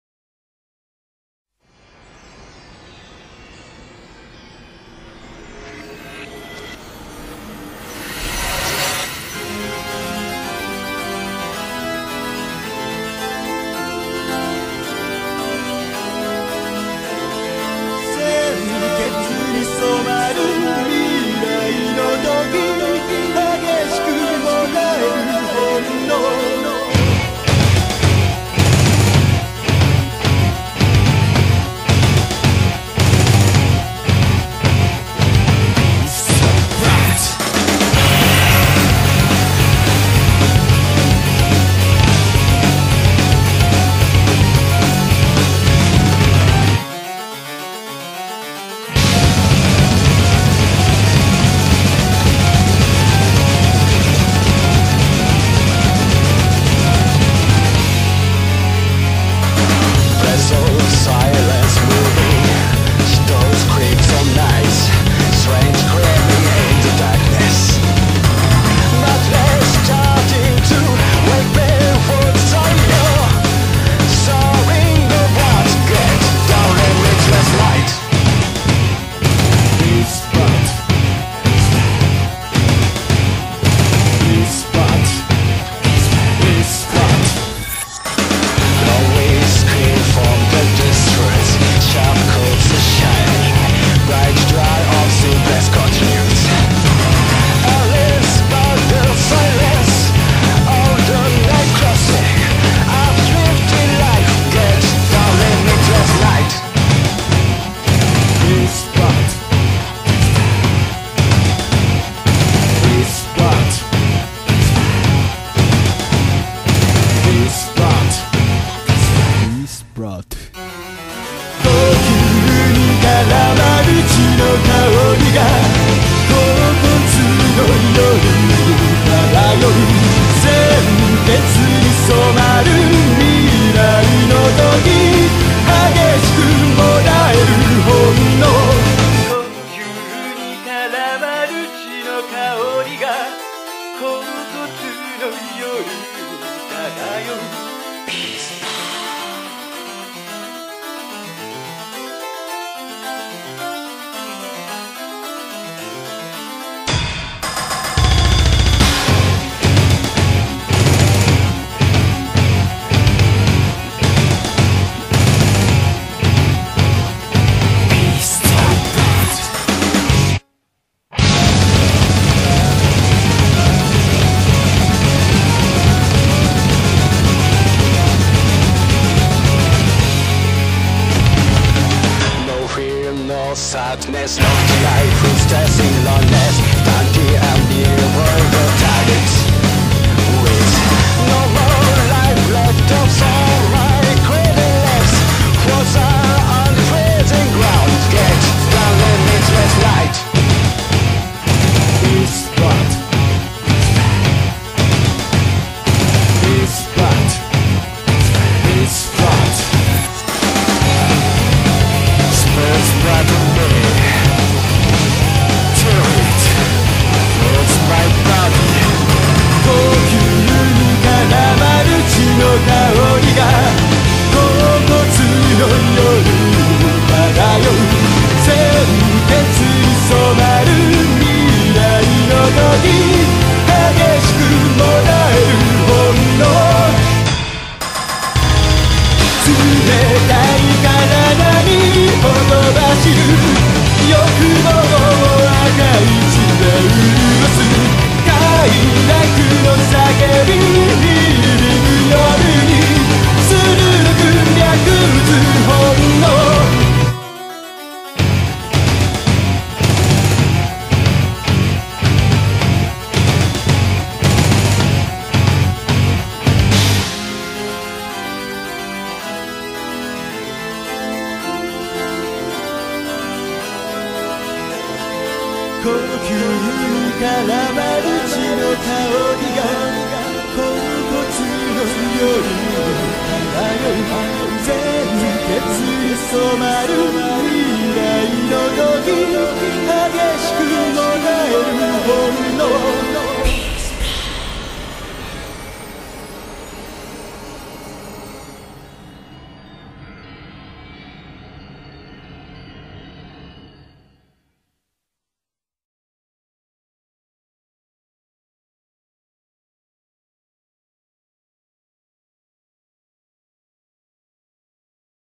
Gothic Lolitas
A collection of Gothic Lolitas. Enjoy, Comment, & Rate! music by Malice Mizer if anyone knows how to get a malice mizer cd in southern california without having to use the net pleeeaaaassseee! tell me.